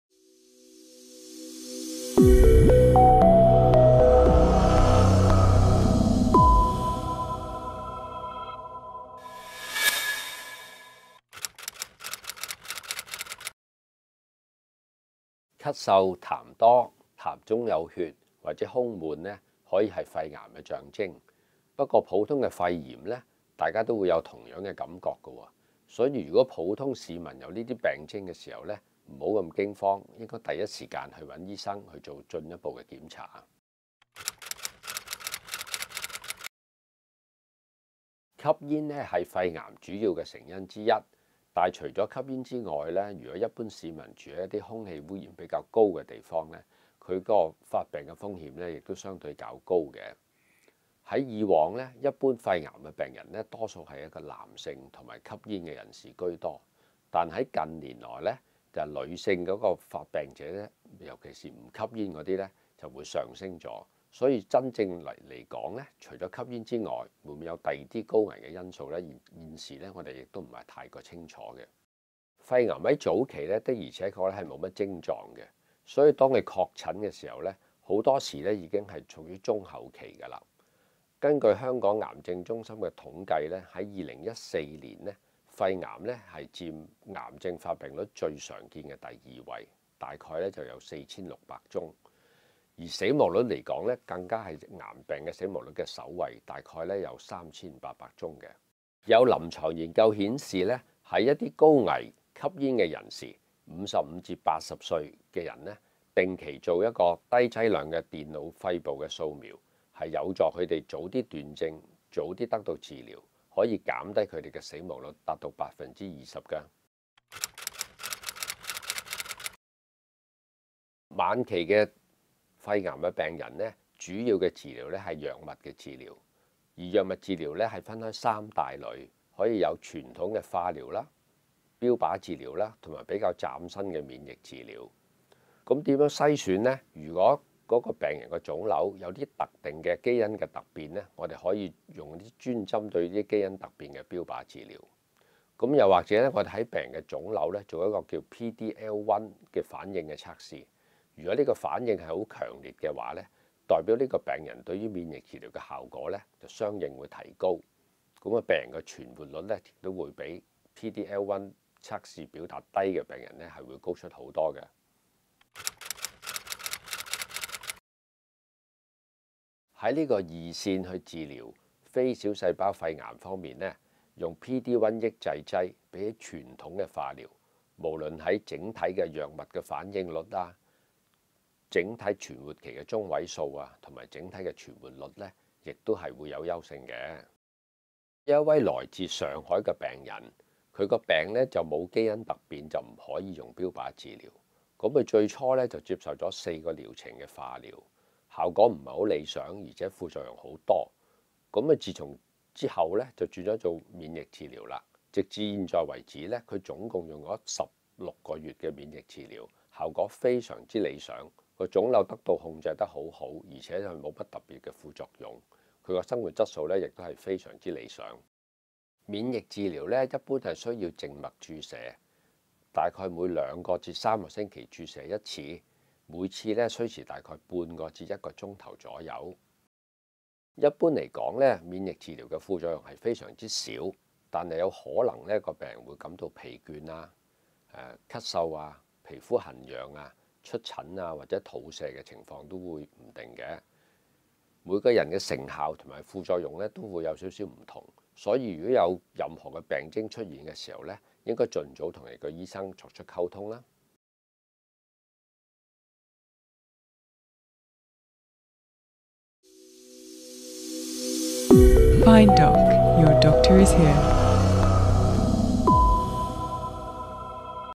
肺癌免疫治療 專題 －
(本短片作健康教育之用，並不可取代任何醫療診斷或治療。治療成效因人而異，如有疑問，請向專業醫療人士諮詢。)

(一) 咳嗽、痰多帶血絲、胸悶，可能是患肺癌？ 0:15

(二) 平時不煙不酒、飲食正常，為何會患肺癌，且是晚期？ 0:38

(三) 確診患上晚期非小細胞肺癌，有什麽治療方案可選擇？ 2:01

(四) 免疫療法治療晚期非小細胞肺癌，療效如何？有副作用嗎？ 2:49